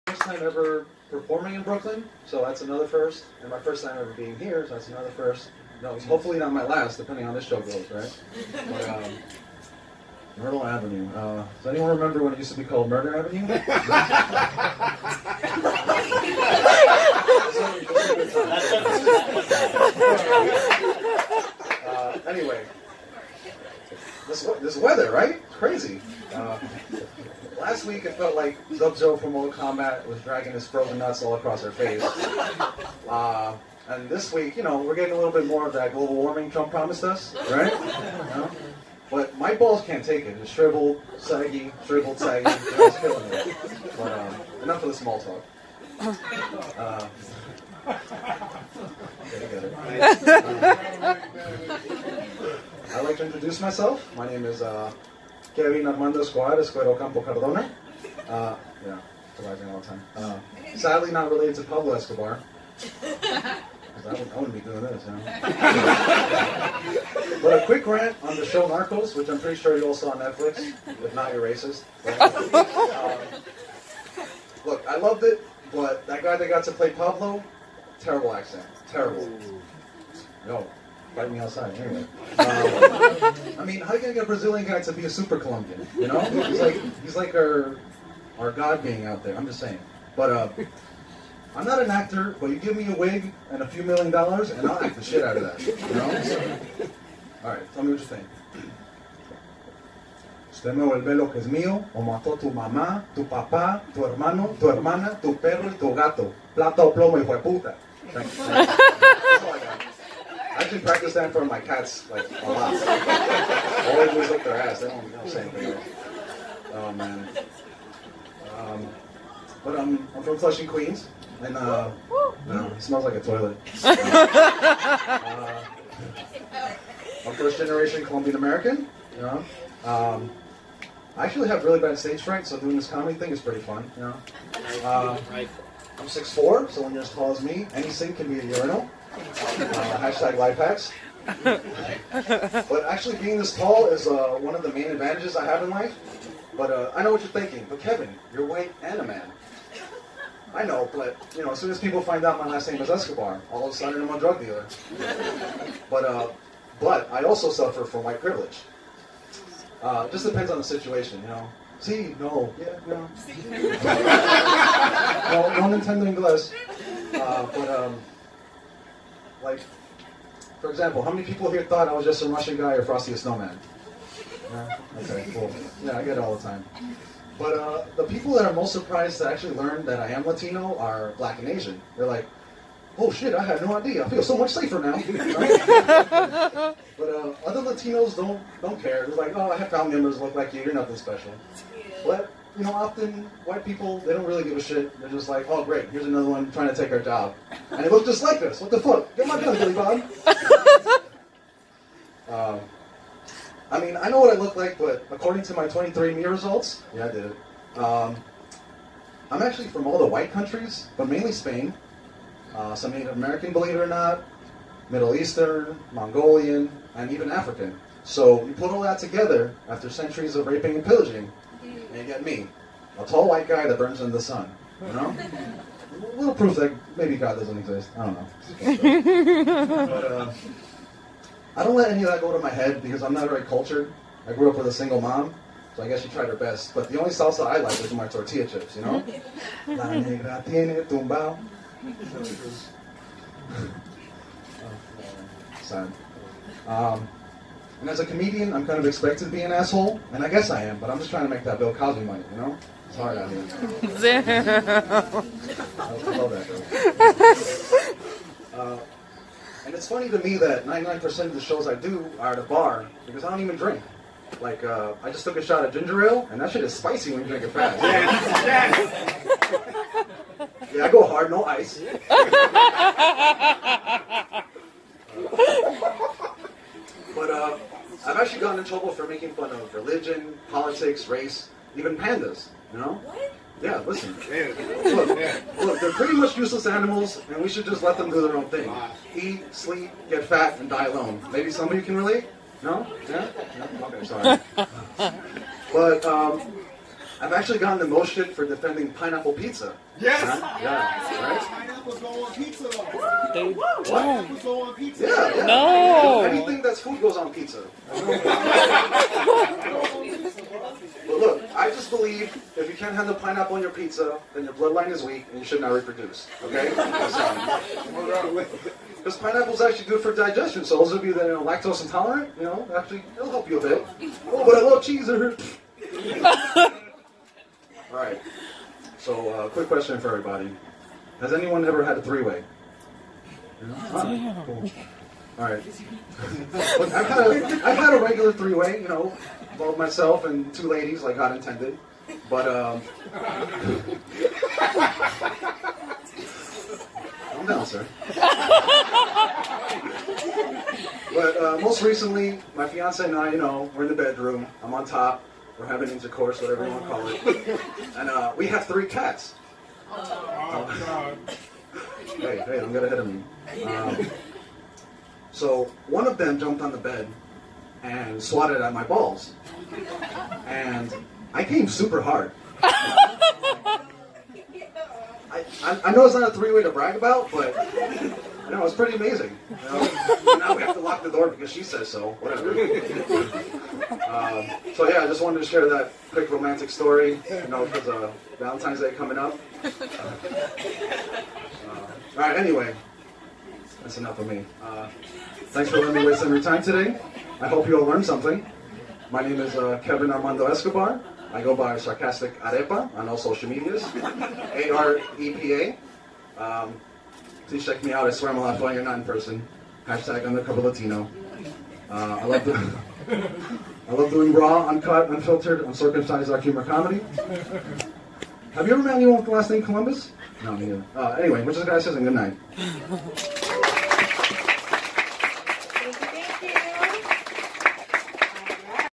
Sarcastic Arepa - standup comedy 2/8/19
I'd like to thank @shaniceariel with @openmicrenegades at the @flowerpowercoffeehousenyc for having me last night. And for @darkntwistyy_ for joining me and also performing. I had a heck of a time! And met some amazing and talented people. and I didn't notice until halfway through my set that there were two little girls and their mom sitting right in front. Oh well, they got to grow up sometime and I guess their mom is just terrible. Idk?